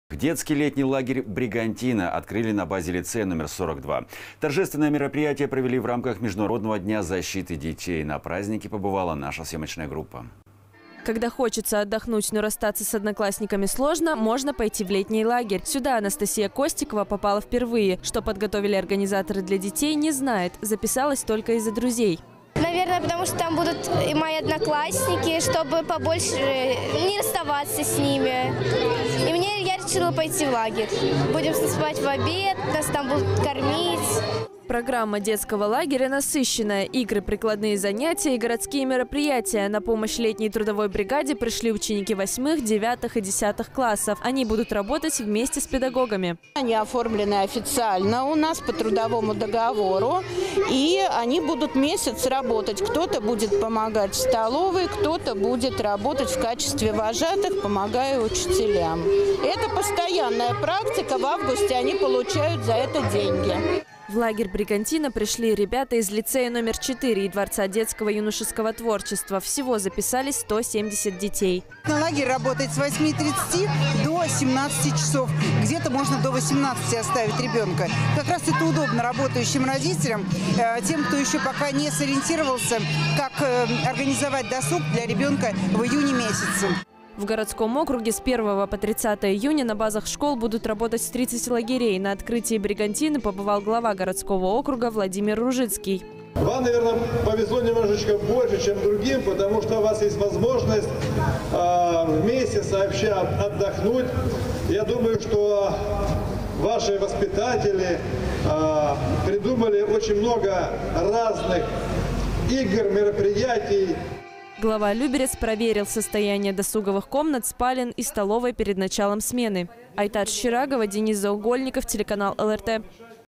Открытие детского летнего лагеря «Бригантина»
Детский летний лагерь «Бригантина» открыли на базе лицея №42. Торжественное мероприятие провели в рамках Международного дня защиты детей. На празднике побывала наша съёмочная группа.

Когда хочется отдохнуть, но расстаться с одноклассниками сложно – можно пойти в летний лагерь. Сюда Анастасия Костикова попала впервые. Что подготовили организаторы для детей – не знает. Записалась только из-за друзей.

Анастасия Костикова, ученица лицея №42 г.о. Люберцы:
«Наверное, потому что там будут мои одноклассники, чтобы побольше… не расставаться с ними, и я решила пойти в лагерь. Будем засыпать в обед, нас там будут кормить».

Программа детского лагеря насыщенная: игры, прикладные занятия и городские мероприятия. На помощь летней трудовой бригаде пришли ученики восьмых, девятых и десятых классов. Они будут работать вместе с педагогами.

Елена Юшко, директор лицея №42 г.о. Люберцы:
«Они оформлены официально у нас, по трудовому договору. И они будут месяц работать, кто-то будет помогать в столовой, кто-то будет работать в качестве вожатых, помогая учителям. Это постоянная практика, в августе они получают за это деньги».

В лагерь «Бригантина» пришли ребята из лицея №4 и Дворца детского юношеского творчества. Всего записались 170 детей.

Виктория Бунтина, начальник управления образованием г.о. Люберцы:
«Лагерь работает с 8:30 до 17 часов, где-то можно до 18 оставить ребёнка. Как раз это будет удобно работающим родителям, тем, кто ещё не сориентировался, как организовать досуг для ребёнка в июне месяце».

В городском округе с 1 по 30 июня на базах школ будут работать 30 лагерей. На открытии «Бригантины» побывал глава городского округа Владимир Ружицкий.

Владимир Ружицкий, глава г.о. Люберцы:
«Вам, наверное, повезло немножечко больше, чем другим, потому что у вас есть возможность вместе сообща отдохнуть. Я думаю, что ваши воспитатели придумали много разных игр, мероприятий».

Глава Люберец проверил состояние досуговых комнат, спален и столовой перед началом смены.